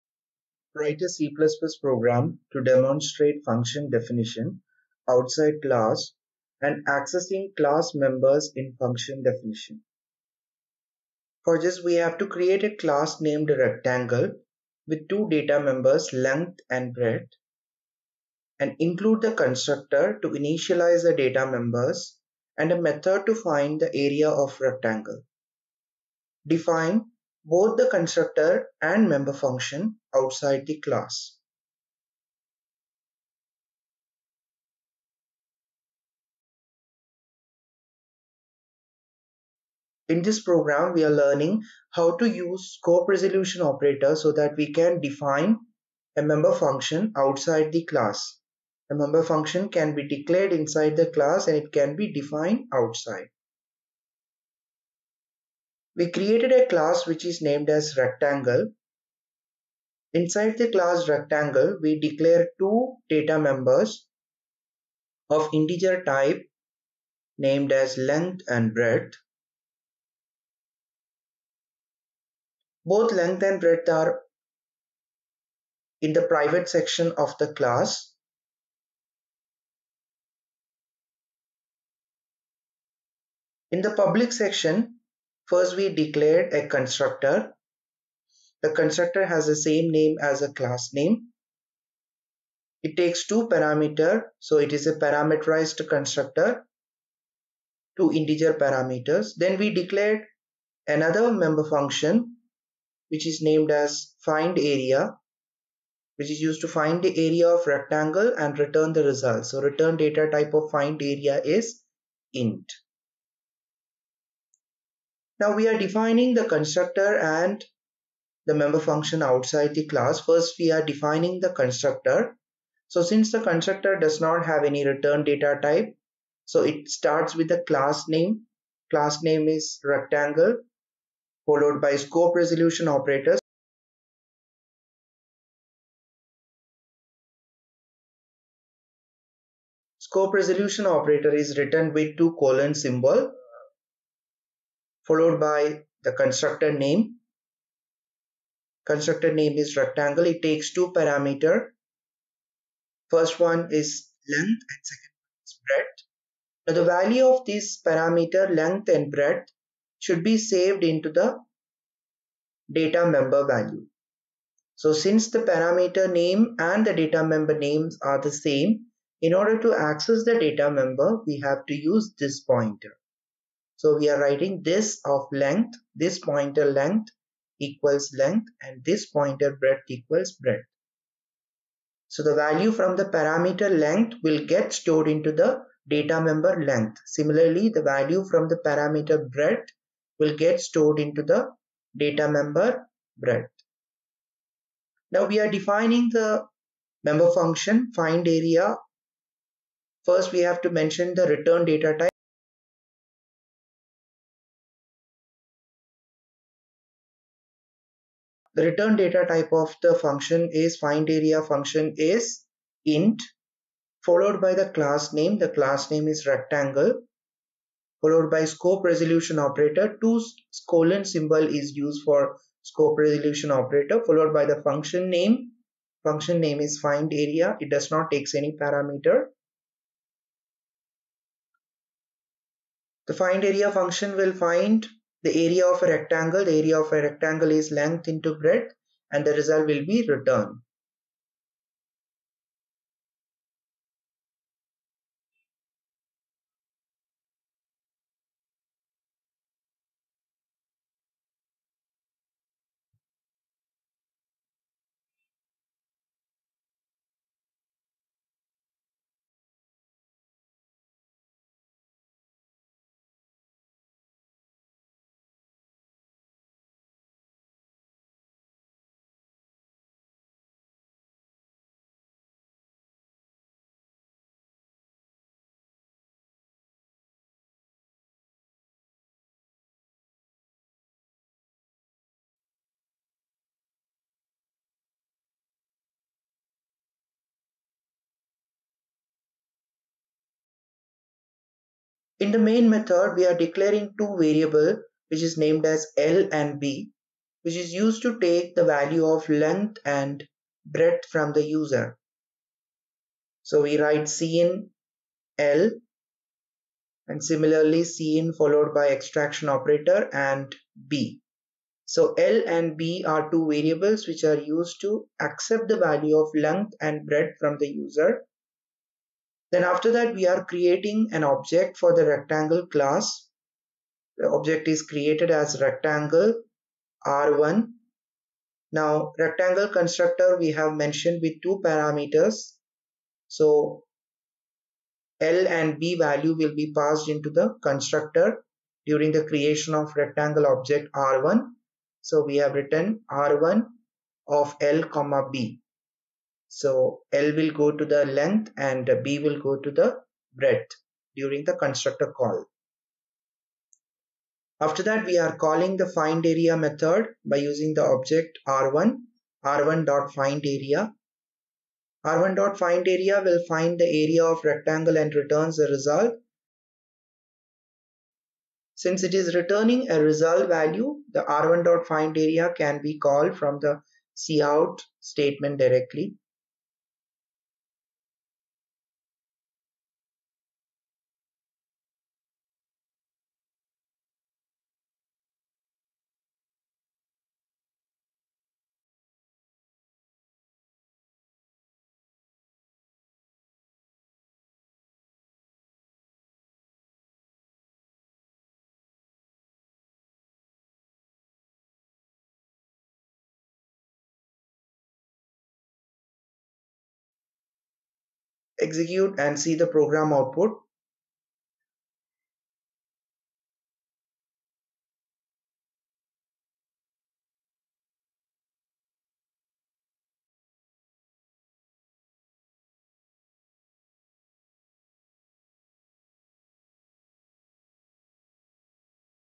C++ Program - scope resolution operator in C++
C++ Program - scope resolution operator in C++  How to define member function outside the class
Defining methods outside the class in C++